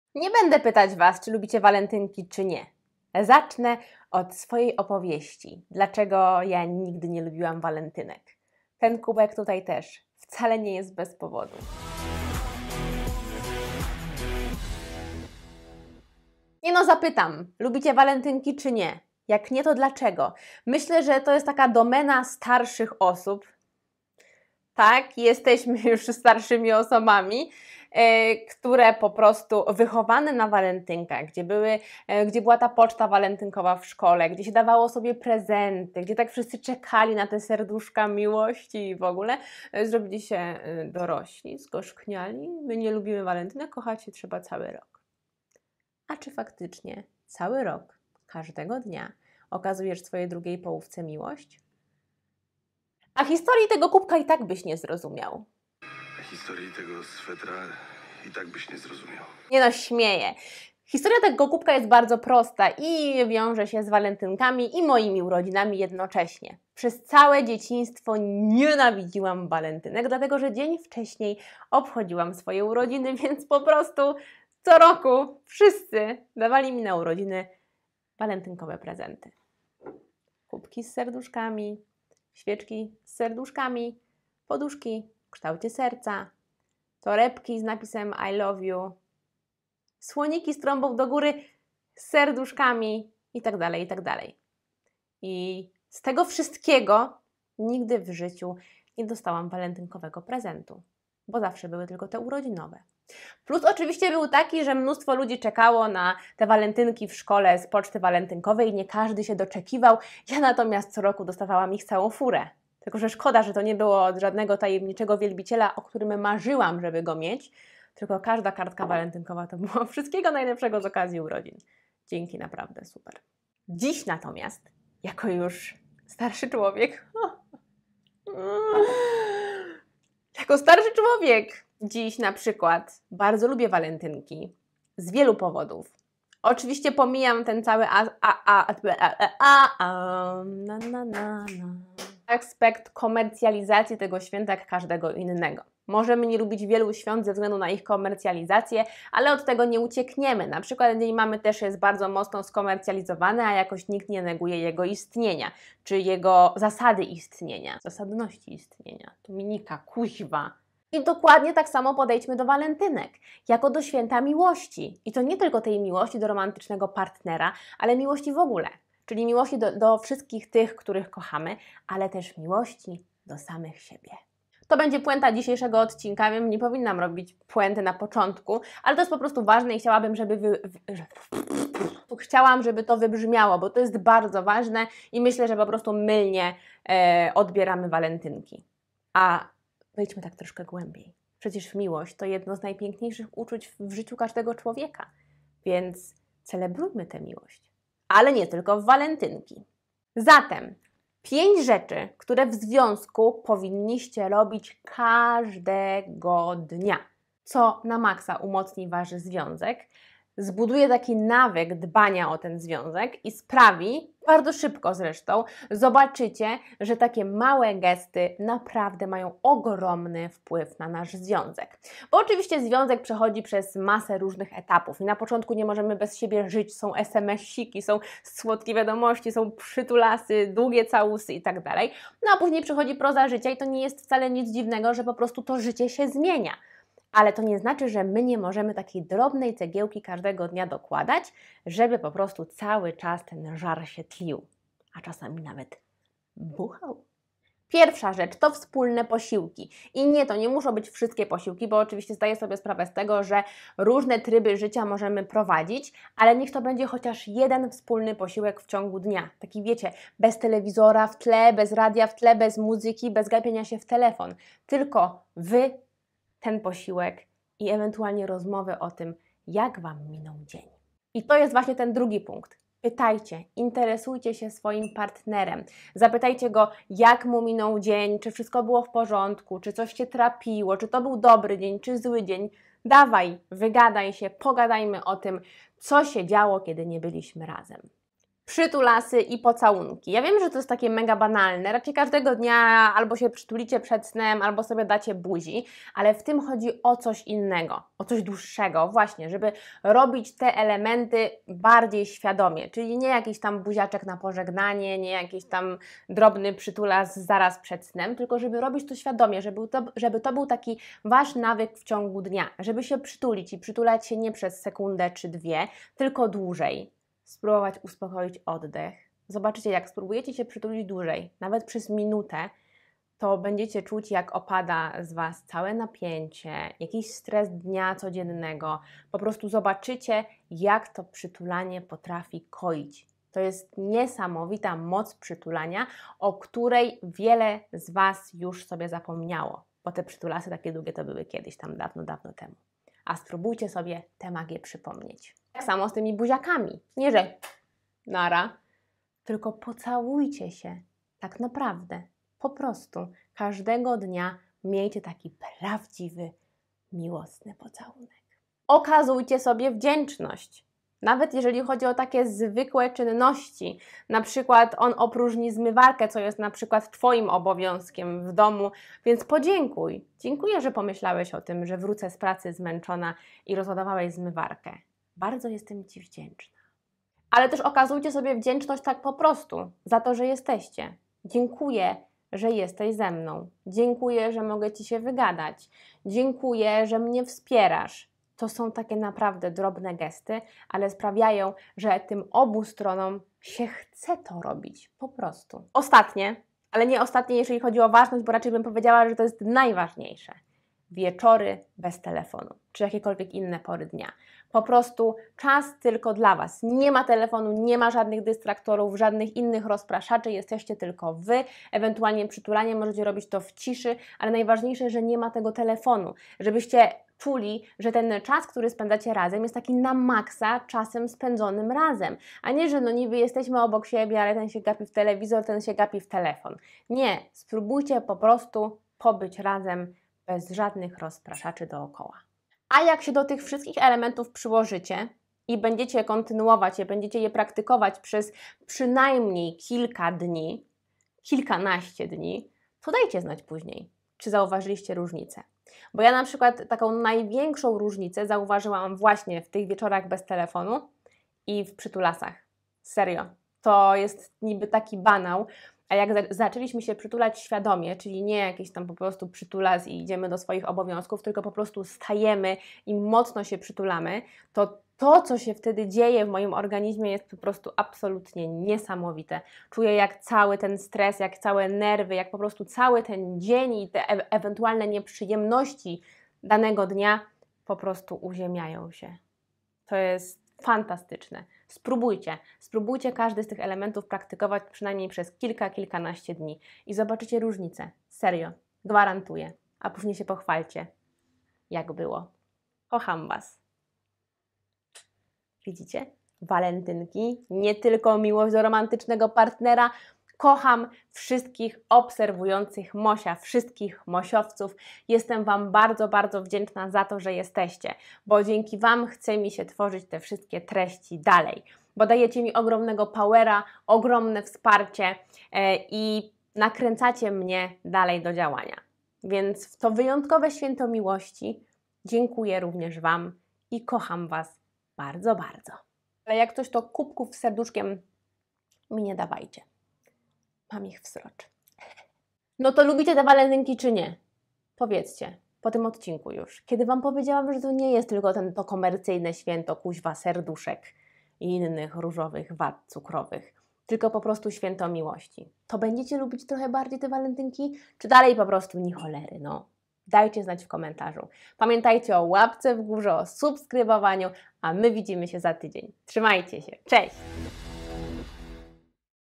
5 rzeczy, które powinniście robić w związku KAŻDEGO dnia | Pomysły | Mówię o Ślubie
Znajdziesz Nas również na Instagram oraz TikTok @mowieoslubie

O związek należy dbać KAŻDEGO dnia. Wdrażając tych pięć drobnych gestów szybko zobaczycie, jak Wasz związek zyskuje na jakości. Nie trzeba wiele, by każdego dnia tlił się między Wami ogień!